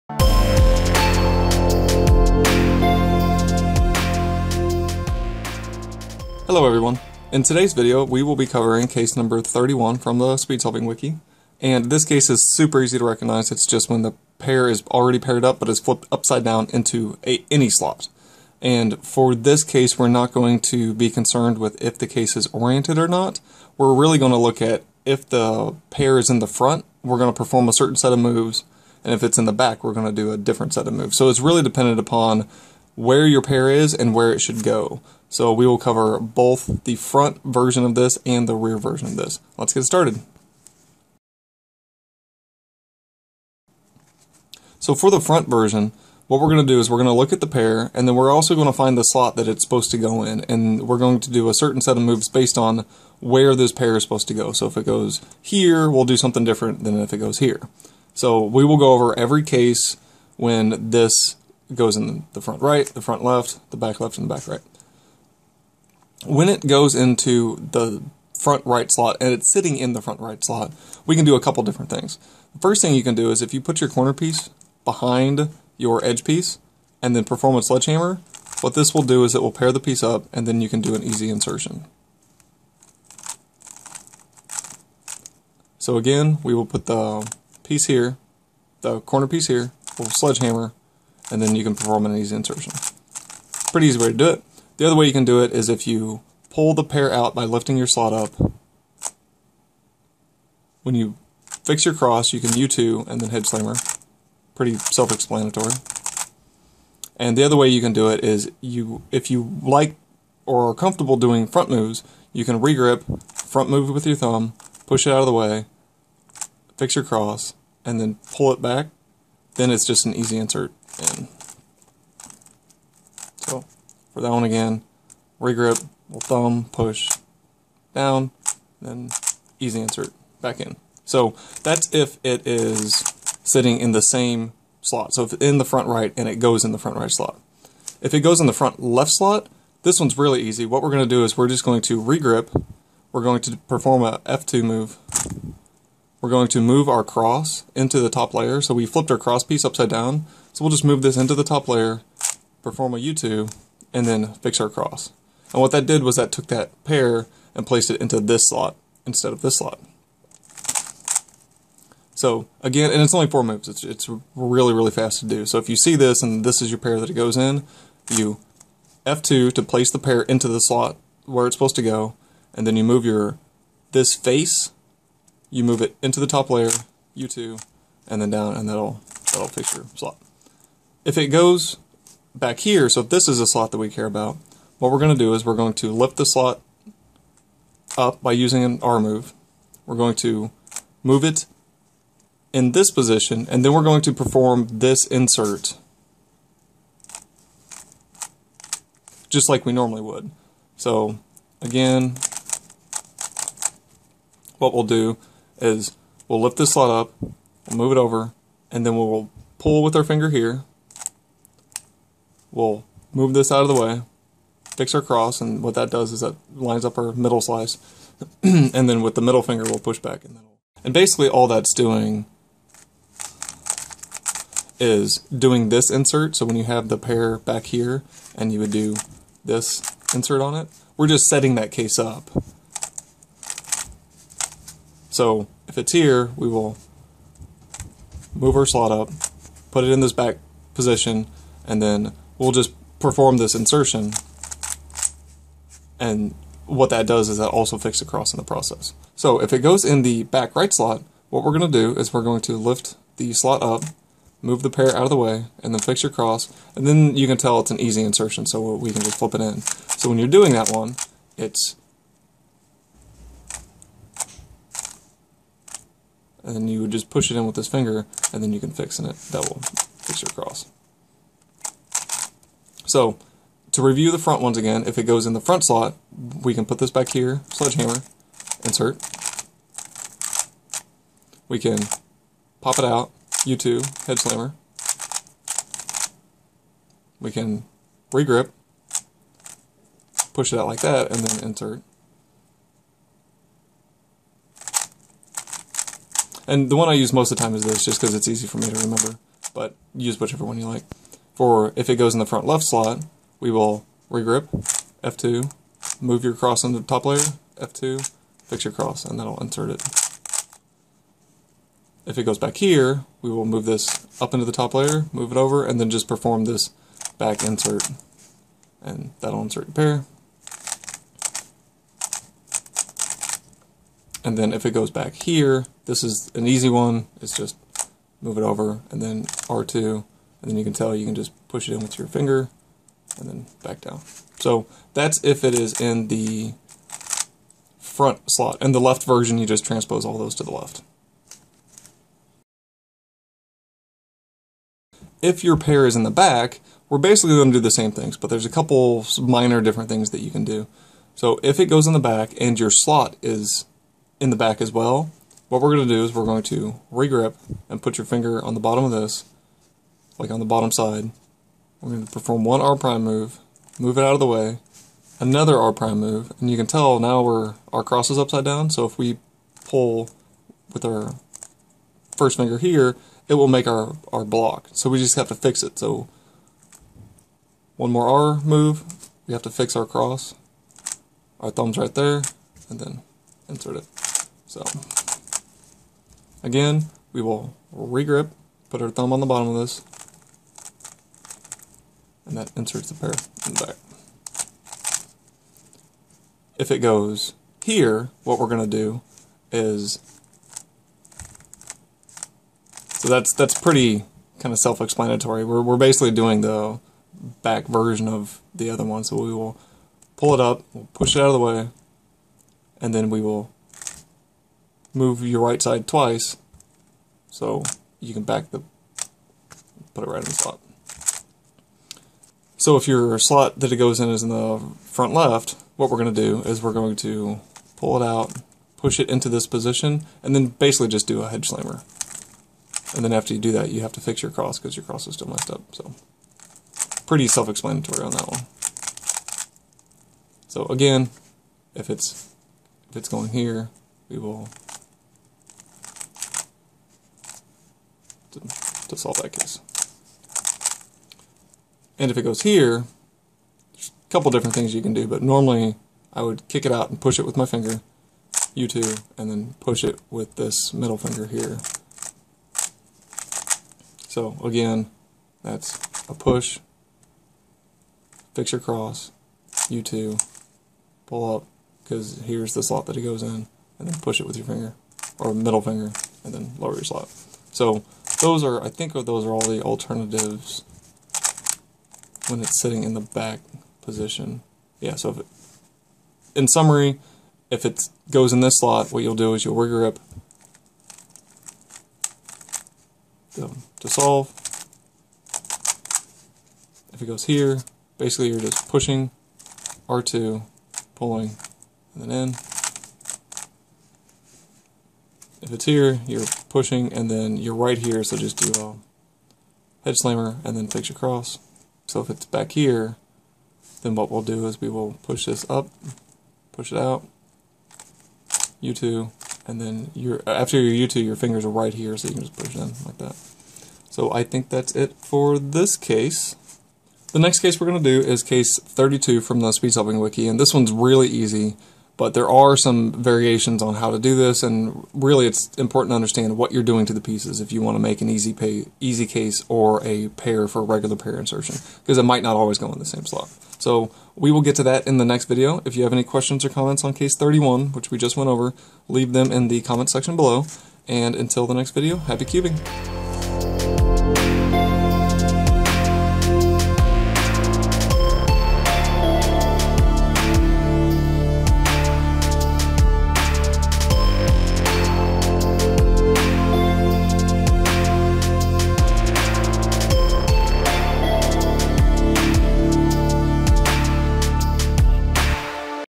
Advanced F2L Case #31
Advanced F2L: Overview of Case #31 from the speed solving wiki

Let me know if you have any questions or comments below.

Happy Cubing! - Caleb Miller.